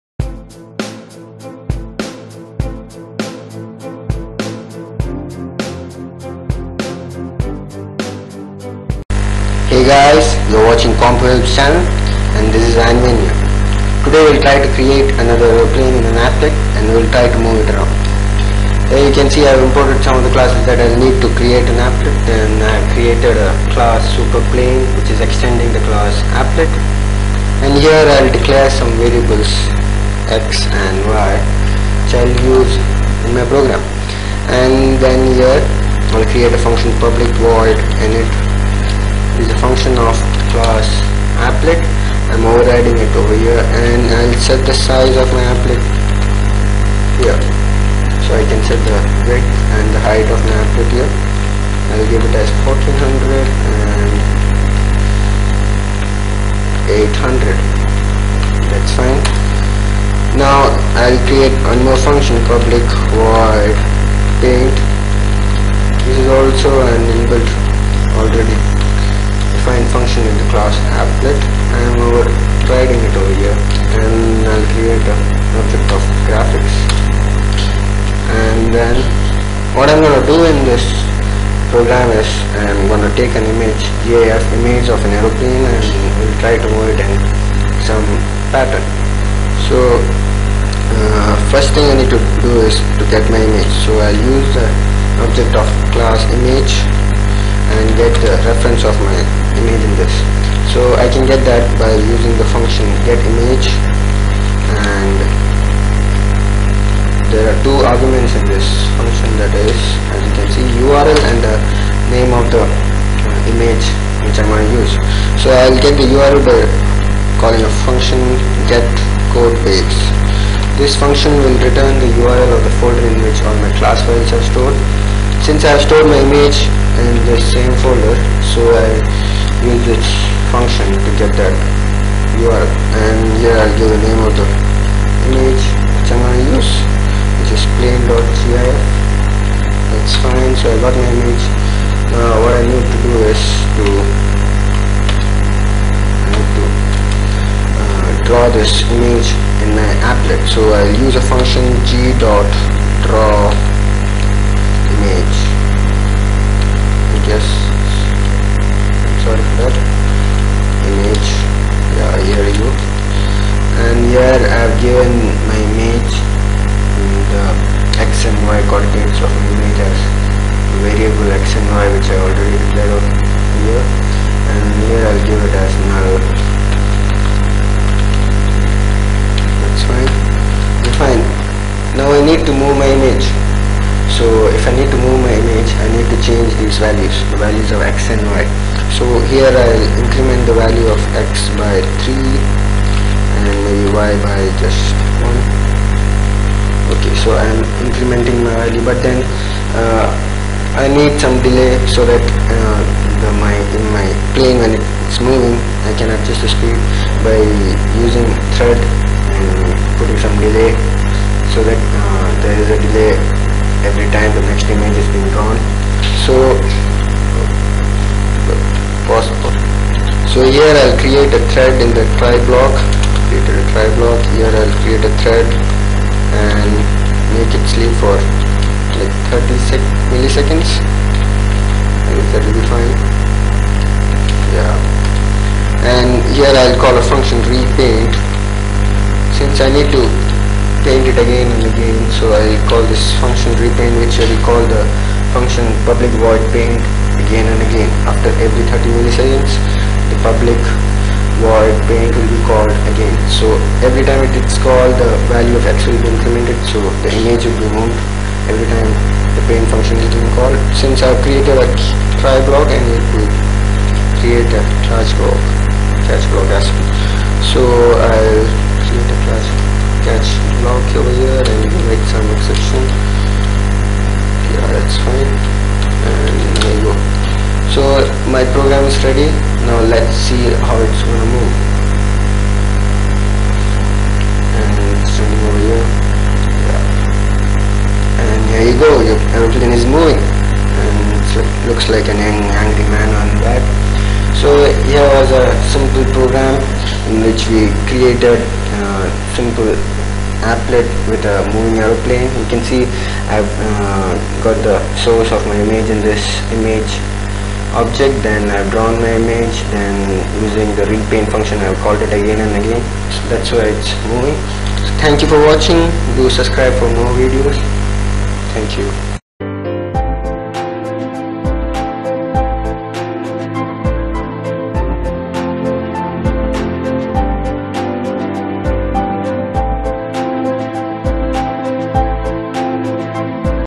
How to create a simple animation in Applet in Java Part-2 ? | CORE JAVA Tutorial
Core Java  tutorial in Hindi.
In this video we are teaching about How to create a simple animation in Applet in Java Part-2?What is animation in Applet in Java? How to create GUI in Java? Use of AWT package in Java? What is Applet life cycle?  Learn Java in easy way in COMPUHELP.